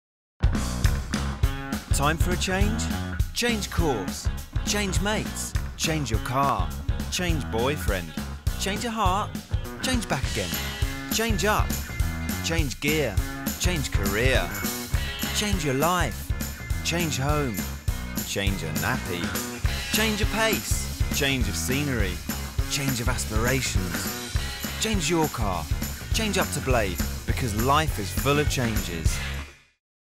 Change up to Blade
Blade group rings in the changes with first ever TV advert!

Every step of the way Blade will be there to provide you with the means to get on the road and stay there - and our new ad reflects exactly this.

So whether you are upscaling or downsizing, starting a family or heading out on your own, or you just fancy a change, Blade will be with you. “Change up to Blade”

now to find out more!